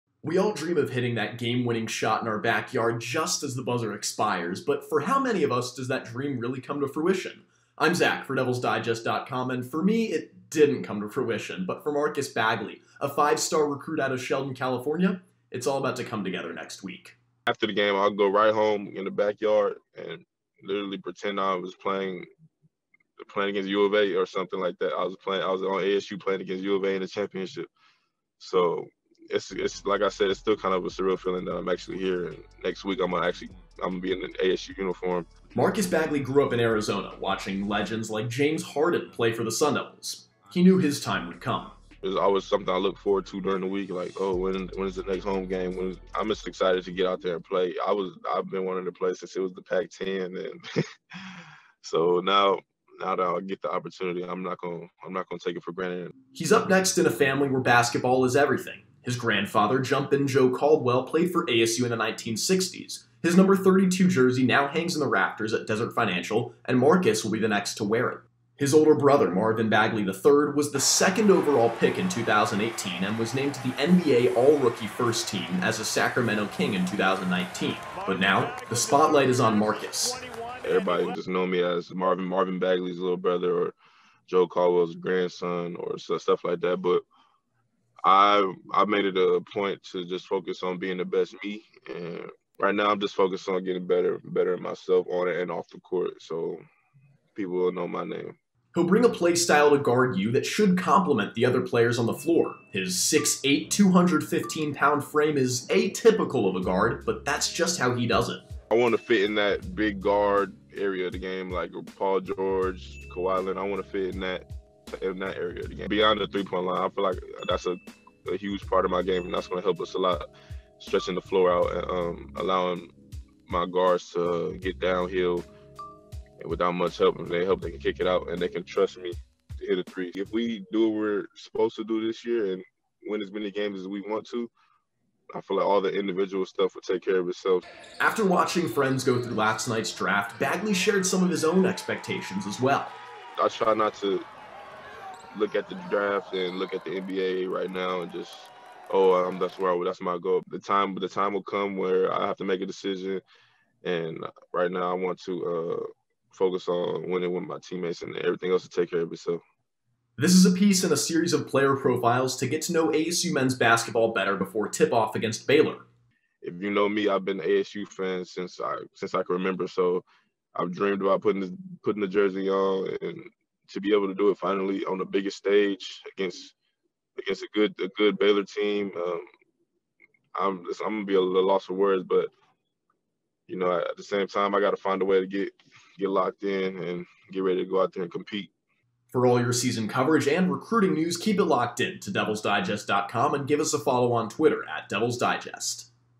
DevilsDigest TV: Player profile - Marcus Bagley determined to make his own name
It's daunting to walk into an arena and see your grandfather's jersey in the rafters. For Marcus Bagley, he's used to living in the shadow of others—including his older brother and NBA player, Marvin. With less than one week until the season tipoff, it's Marcus' turn to shine.